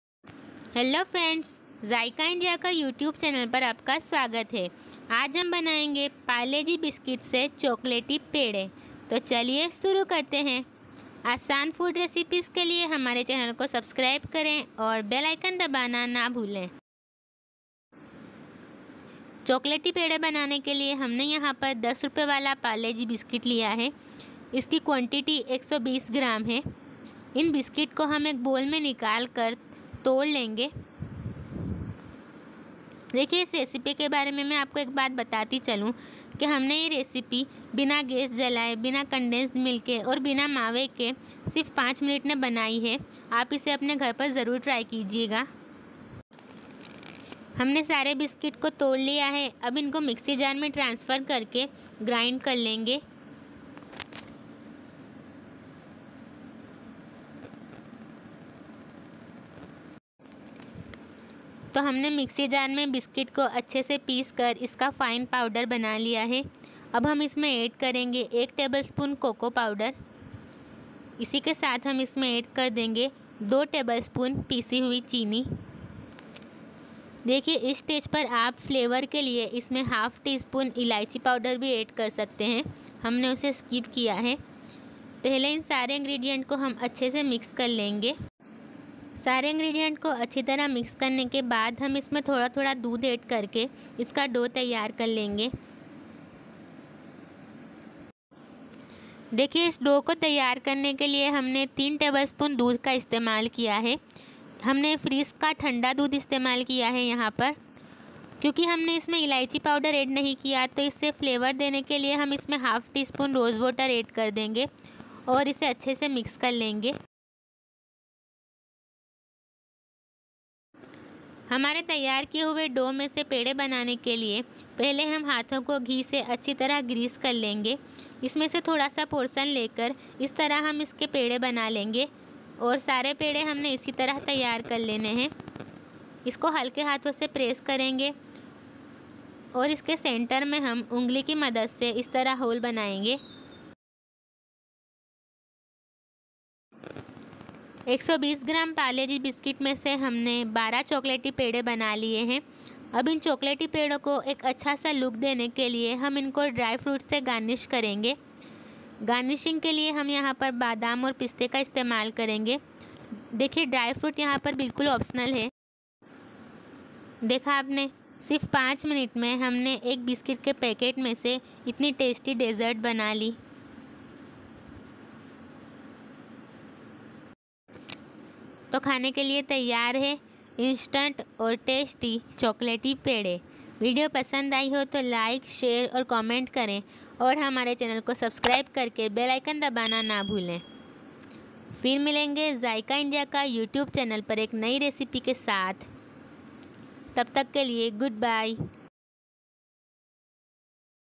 Chocolate Peda Recipe | How to make Chocolate Peda | चॉकलेट पेडा कैसे बनाये | Instant Sweet
How to make chocolate peda at home,
Instant recipe for making chocolate peda,
5 minutes recipe